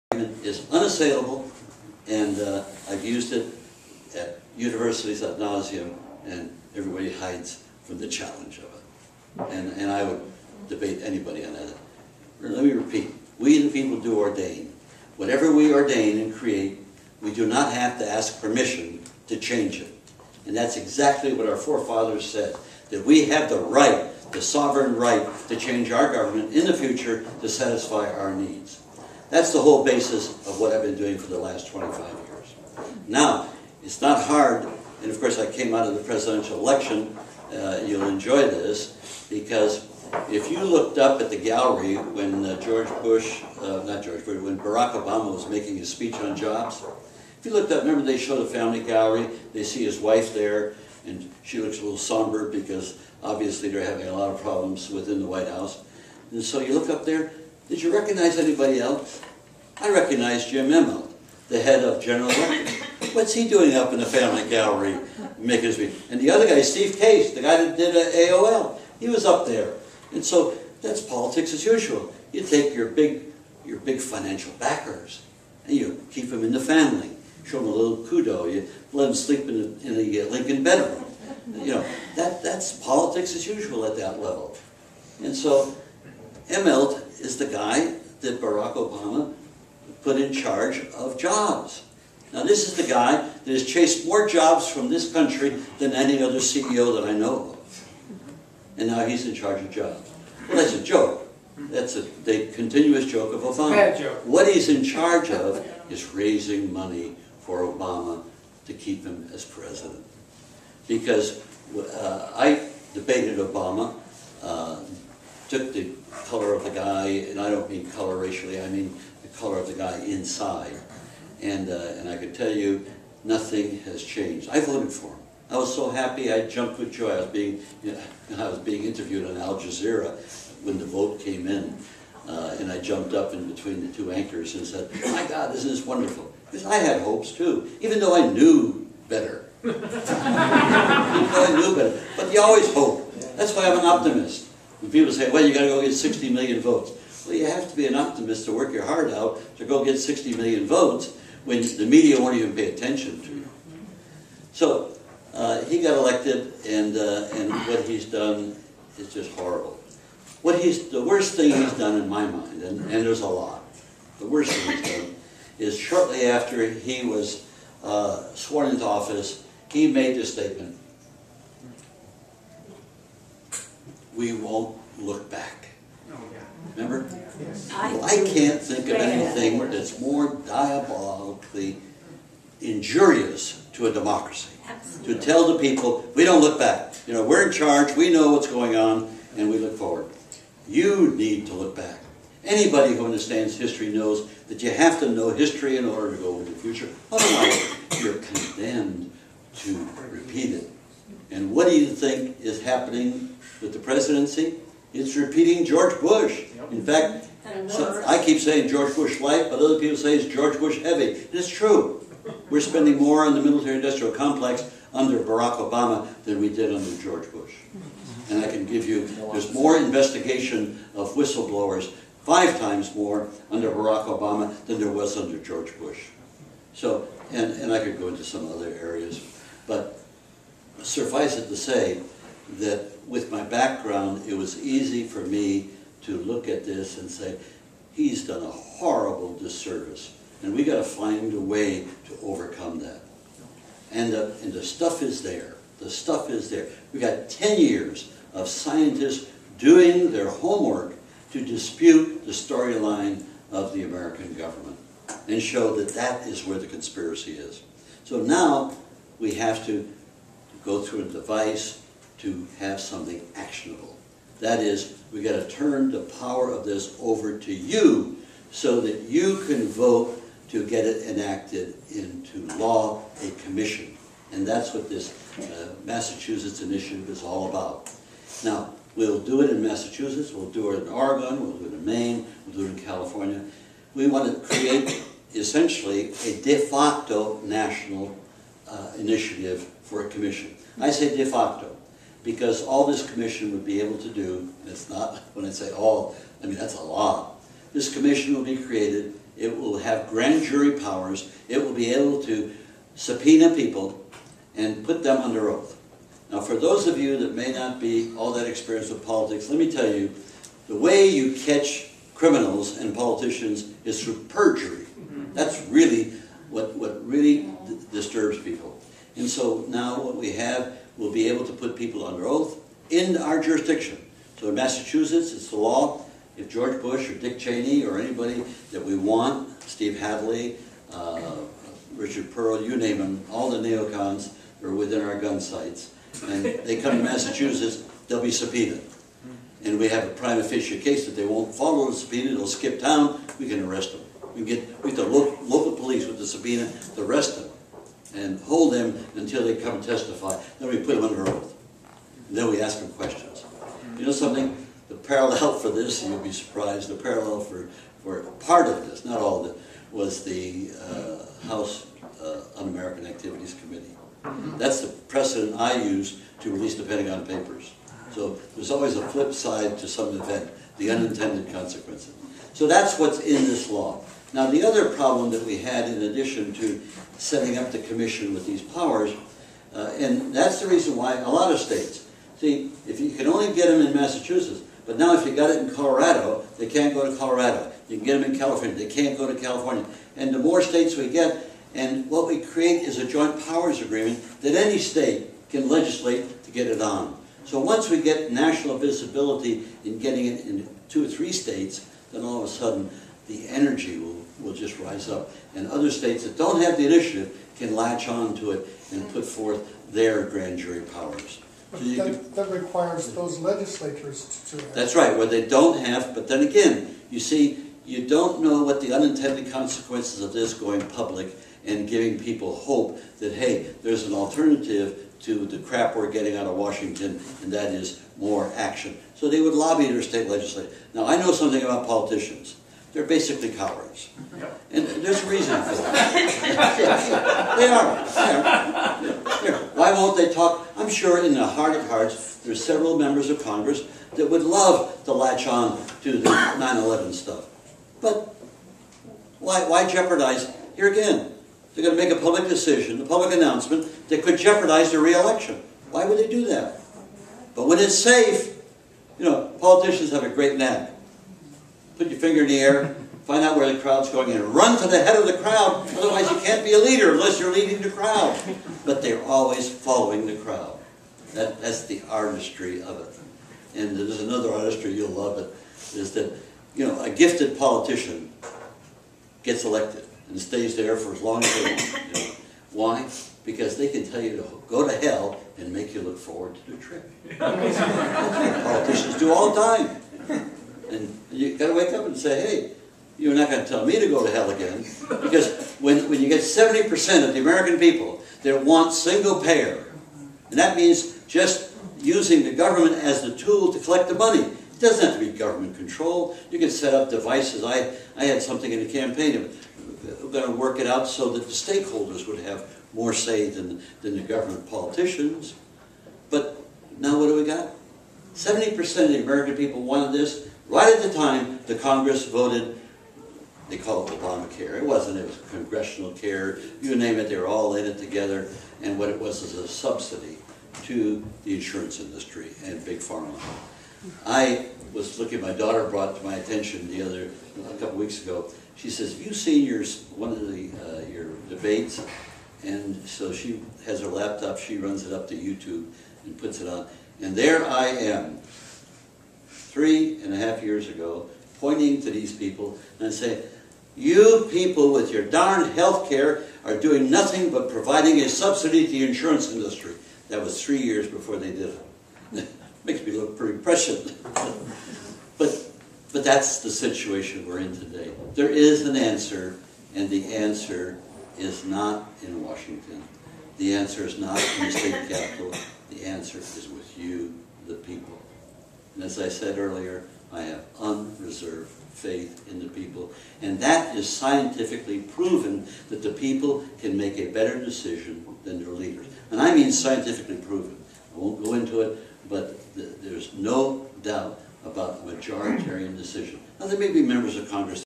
Mike Gravel 4/6 on the Citizens' 9/11 Investigation Commission
Recorded on September 20, 2011 using a Flip Video camera at Media Education Foundation in Northampton, MA